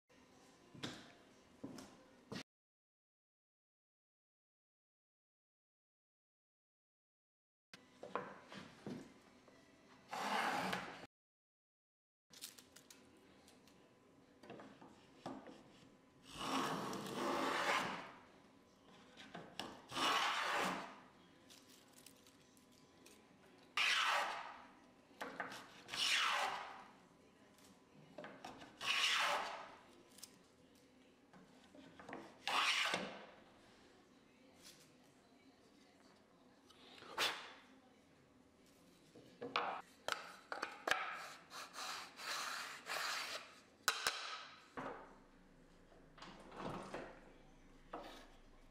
Making Drawer Grooves with a Grooving Plane
Using an 1800's restored grooving plane to make a drawer groove. An alternative methos to create drawer bottom grooves. Easy to set up , dust-free, noise-free. In the past I would not hesitate to perform this operation with a router, but with a properly sharpened and st up grooving plane, the process is far more tactile and enjoyable.